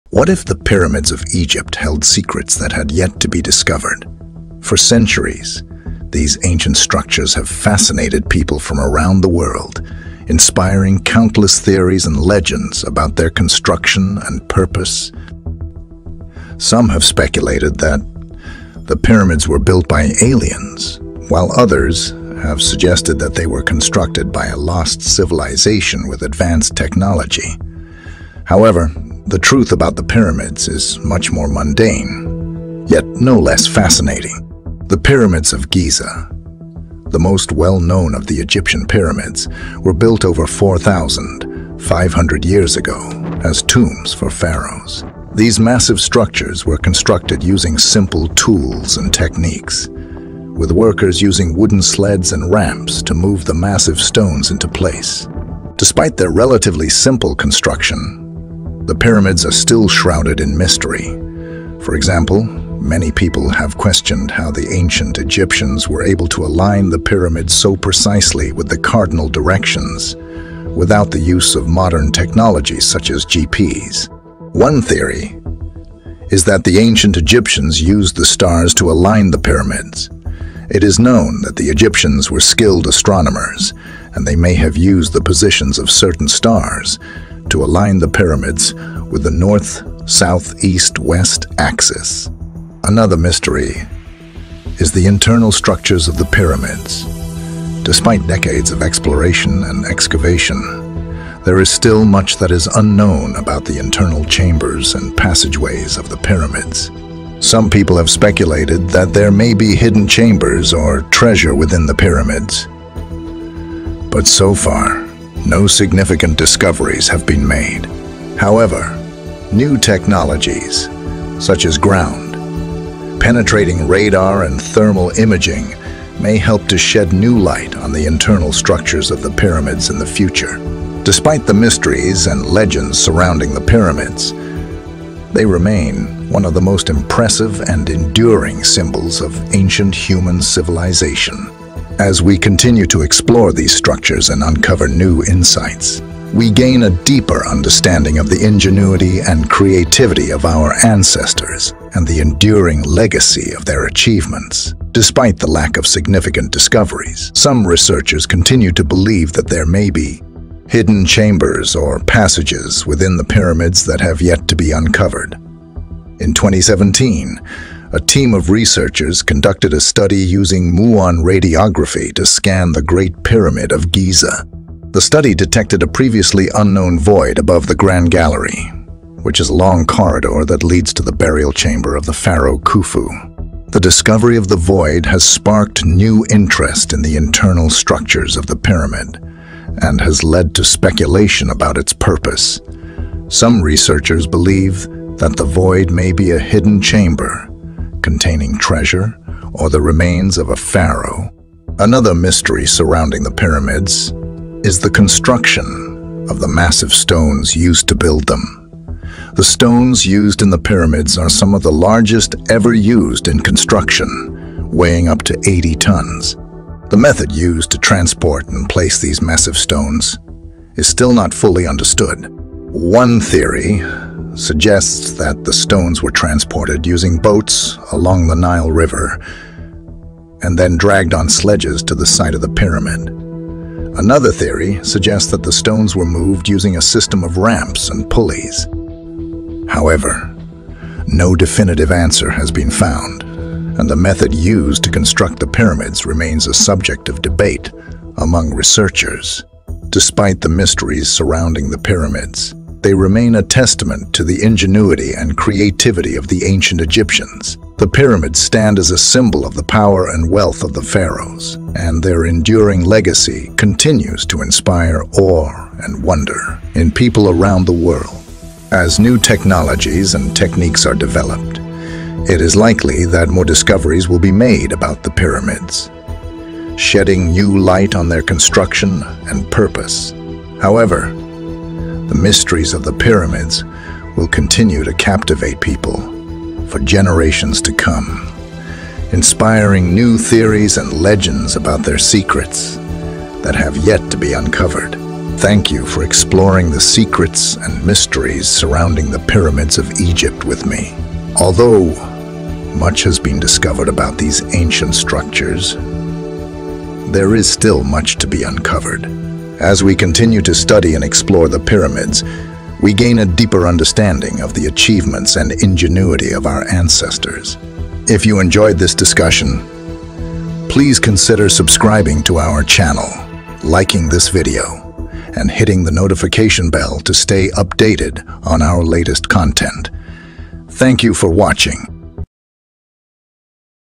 Ancient Builders of the Pyramid
Join us on a journey to explore the secrets and mysteries surrounding the Pyramids of Egypt.